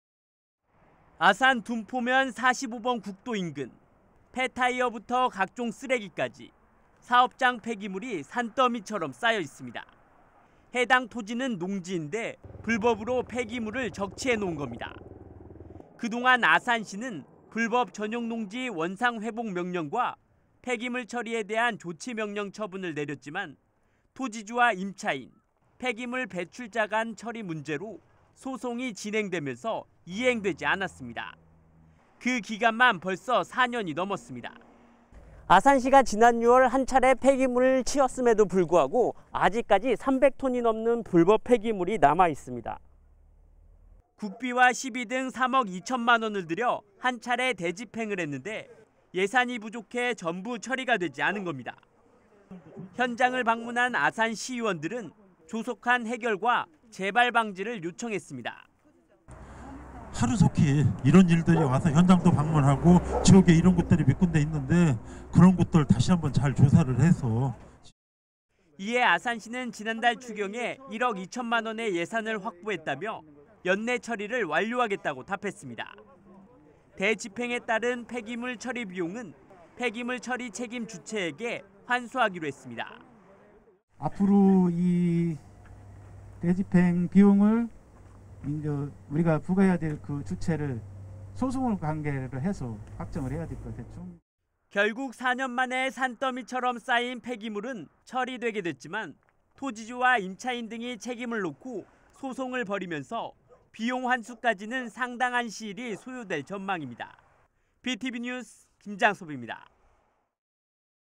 [Btv 중부뉴스]아산 둔포 '산더미 폐기물' 연내 처리 된다
아산 둔포면에 수년 째 쌓여 있는 사업장 폐기물을 놓고

아산시의회가 조속한 처리를 촉구했습니다.
이에 아산시는 연내 폐기물 처리를 완료하겠다고 답했습니다.
보도에 김장섭 기자입니다.